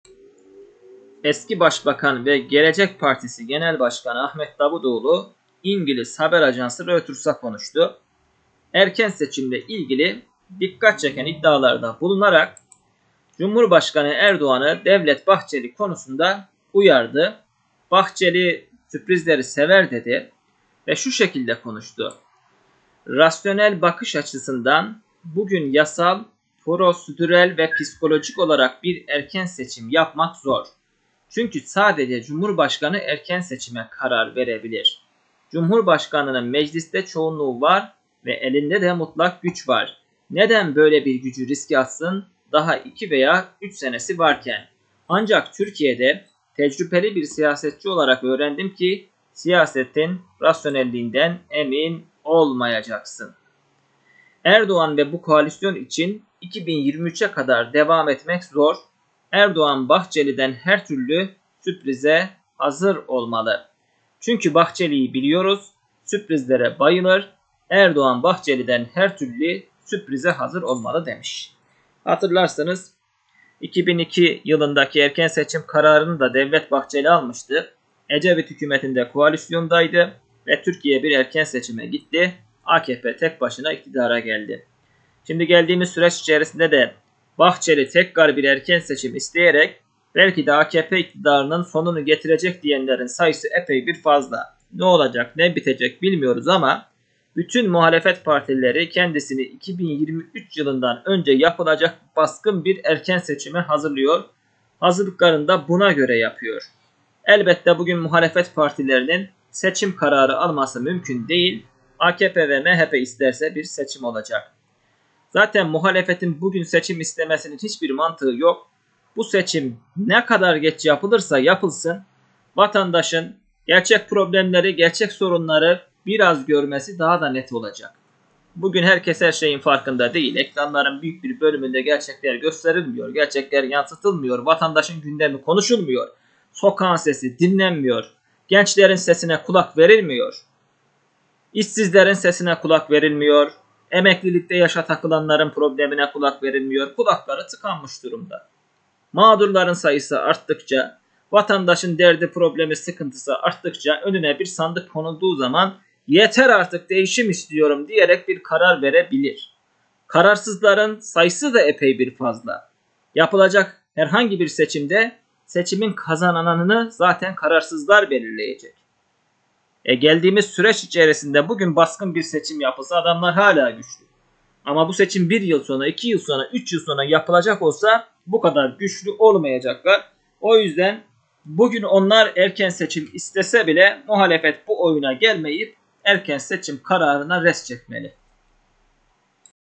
Davutoğlu yaptığı son açıklamayla Ankara gündemini sarstı şimdi ne olacak..
Gelecek Partisi Genel Başkanı Ahmet Davutoğlu erken seçim iddiasıyla gündemi sarstı. Yaptığı açıklama ezber bozdu.
Erken seçim geliyor mu